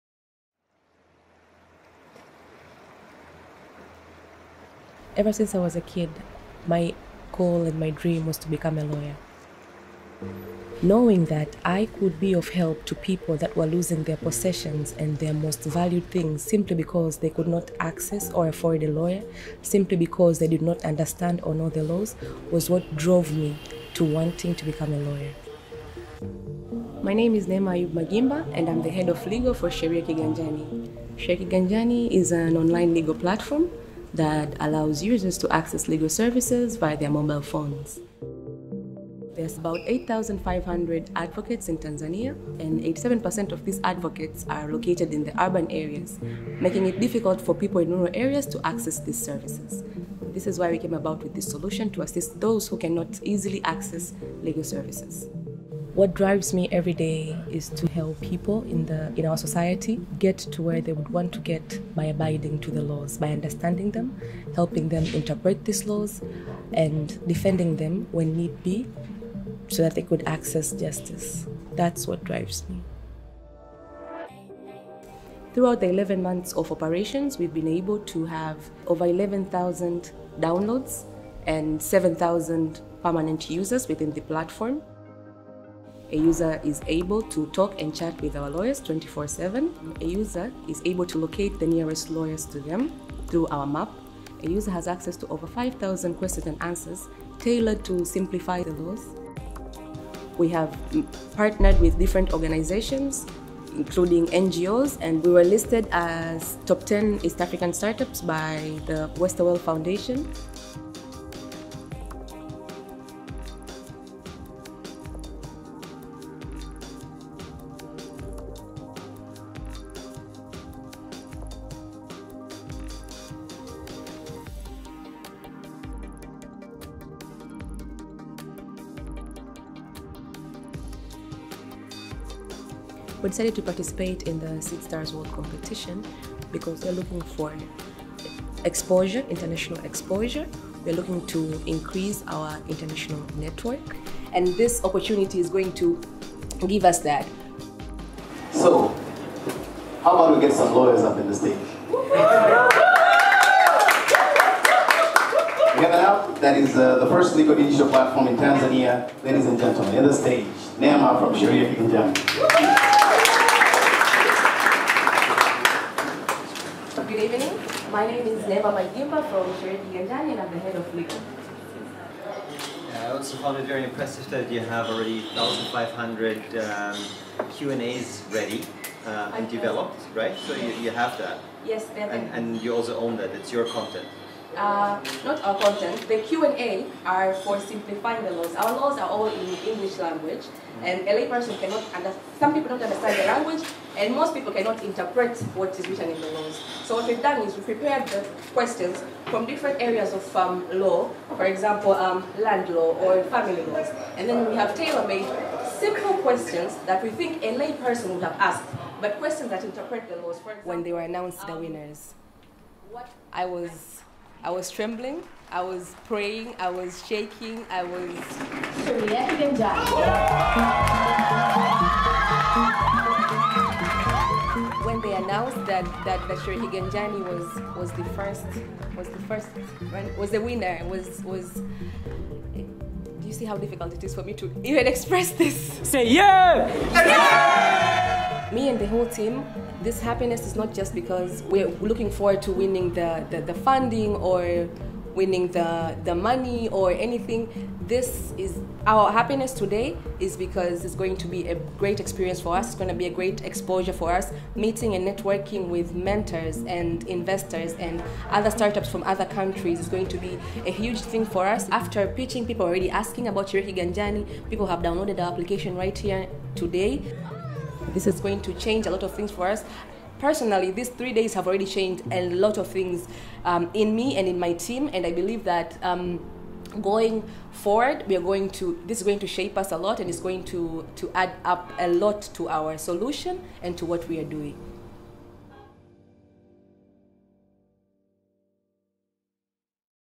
Neema: Personal Lawyer in Your Pocket | Entrepreneur Spotlights
Bringing the law to the palm of your hand, Sheria Kiganjani was created to give people access to various legal services and information via their mobile phones. Meet Neema Yubmagimba, Sheria Kiganjani's Head of Legal, whose drive as a lawyer is helping people attain legal help and knowledge. And through Sheria Kiganjani, she has been able to do just that for the Tanzanian people as their platform enables easy access to legal information and justice. More videos like this